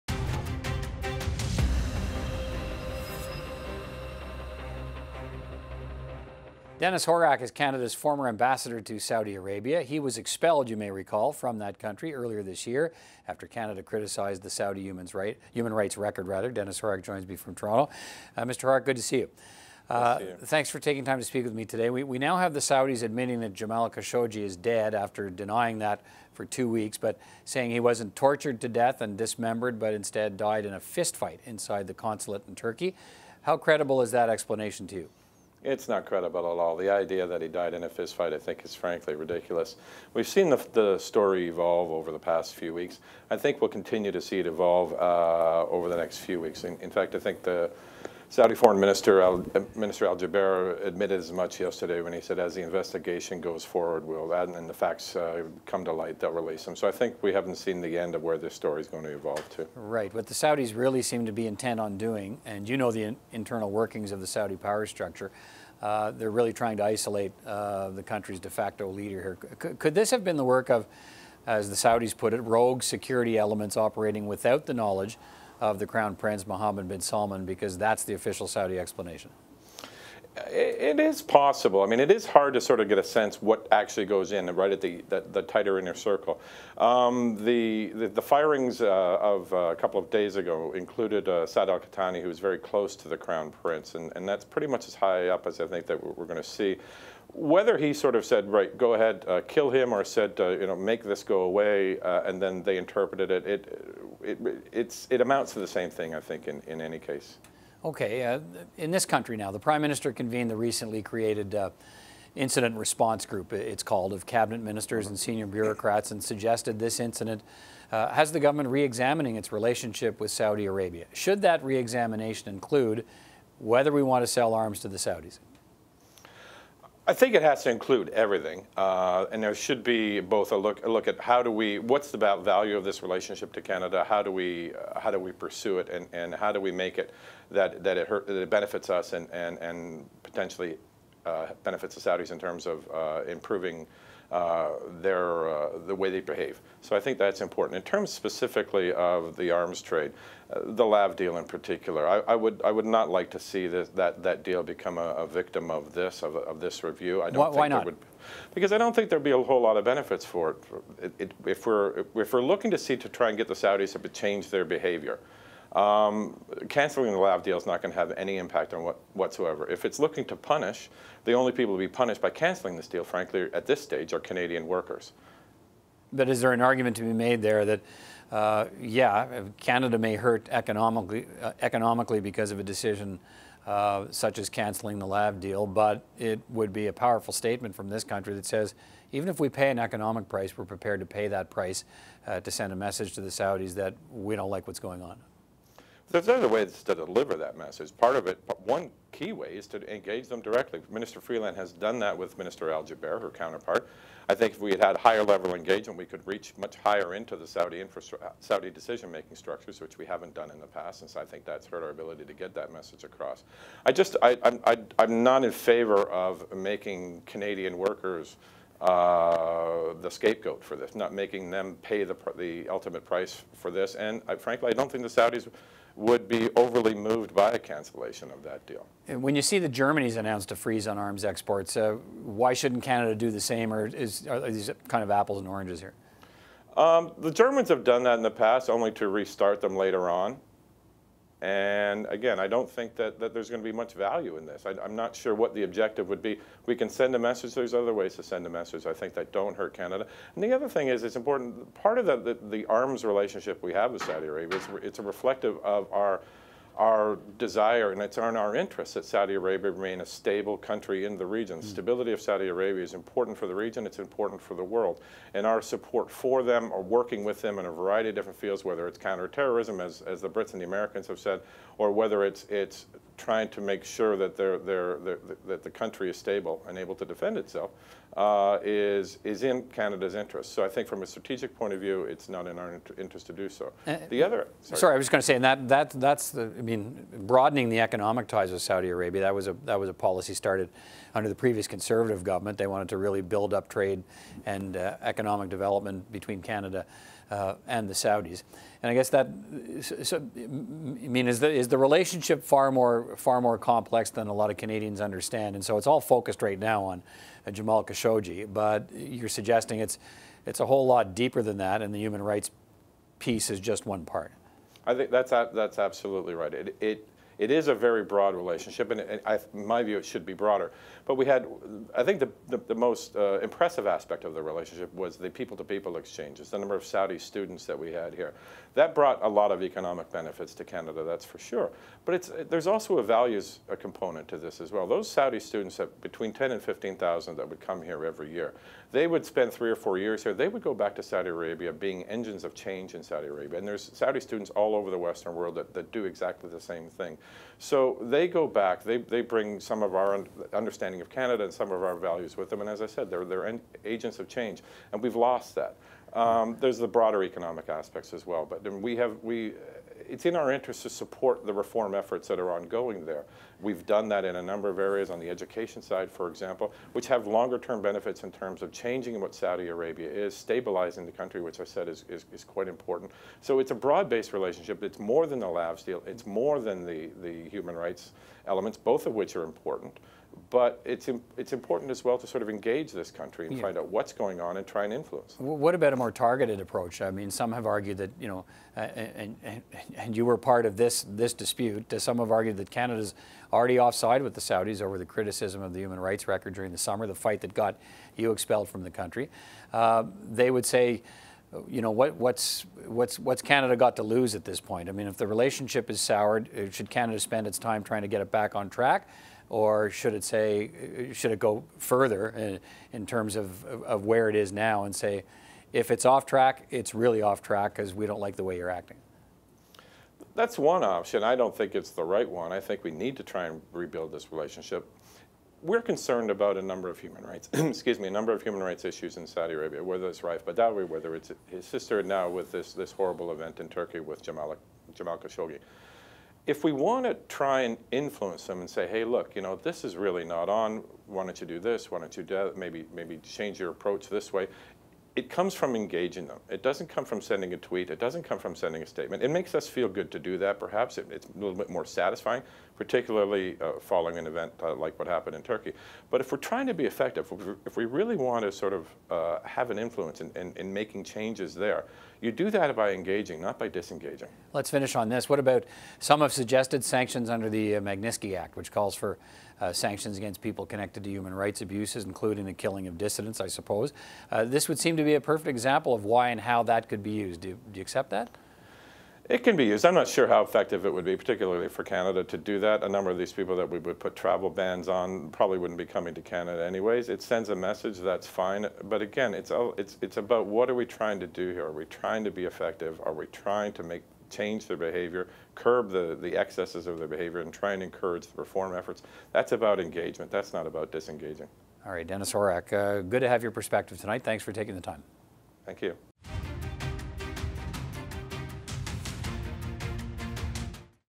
Former ambassador to Saudi Arabia weighs in after Khashoggi killing
Canada's former ambassador to Saudi Arabia, Dennis Horak, joins CPAC's Peter Van Dusen to discuss Canada's diplomatic response to Saudi Arabia's killing of journalist Jamal Khashoggi.